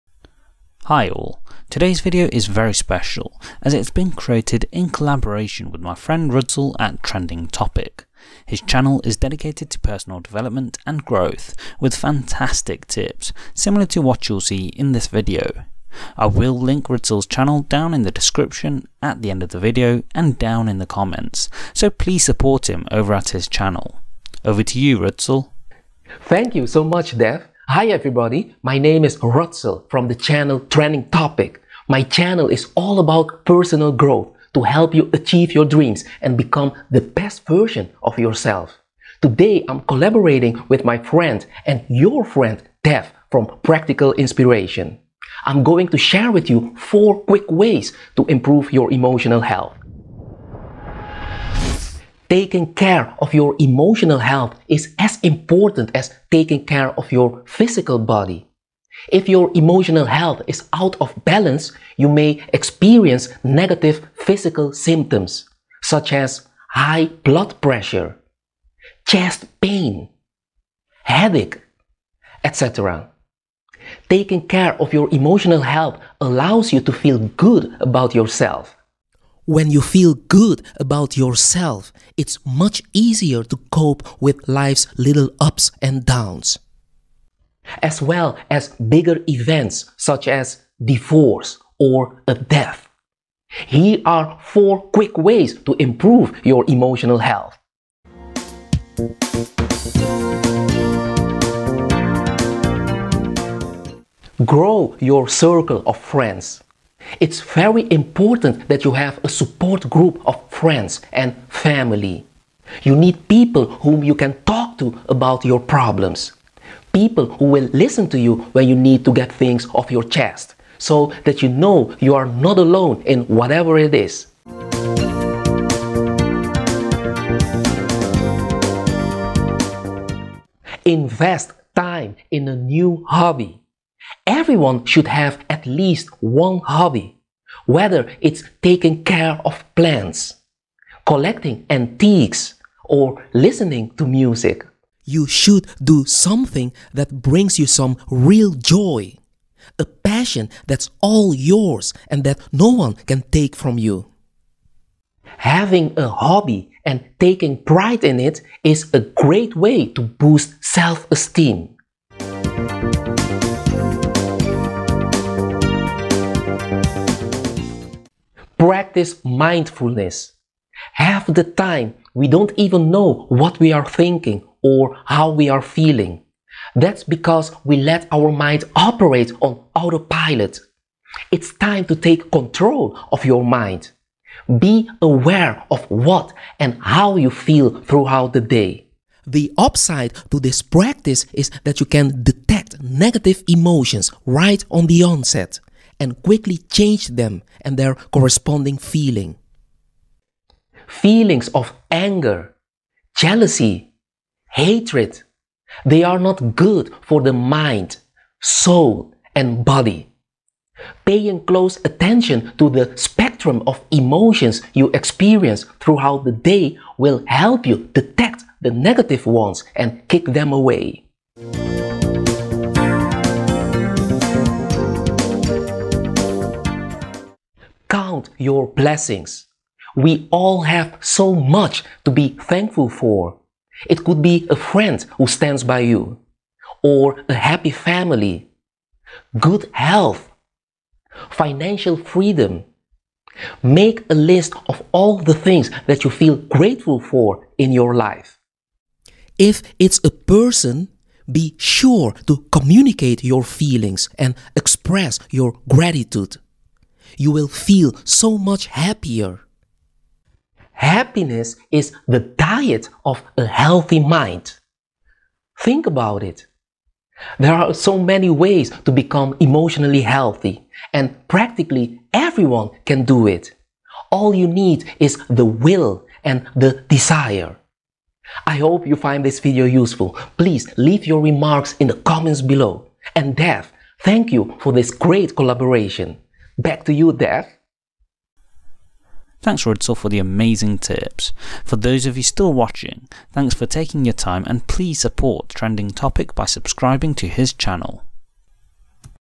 How To Improve Emotional Health | How To Be Mentally Healthy And Happy
If you want to learn How To Improve Emotional Health and How To Be Mentally Healthy And Happy by taking care of your emotional health, then this video on Practical Inspiration, in collaboration with Trending Topic, is for you. Caring for your emotional and mental health is fundamental in life and is of vital important.

Here, we provide tips for promoting good mental health, as you learn how to Improve Emotional Health and Be Mentally Healthy. So Be Mentally Healthy and happy and learn 4 amazing tips on how to improve your mental and emotional health. Use these Tips to improve your mental and emotional health and learn how to be happy and confident.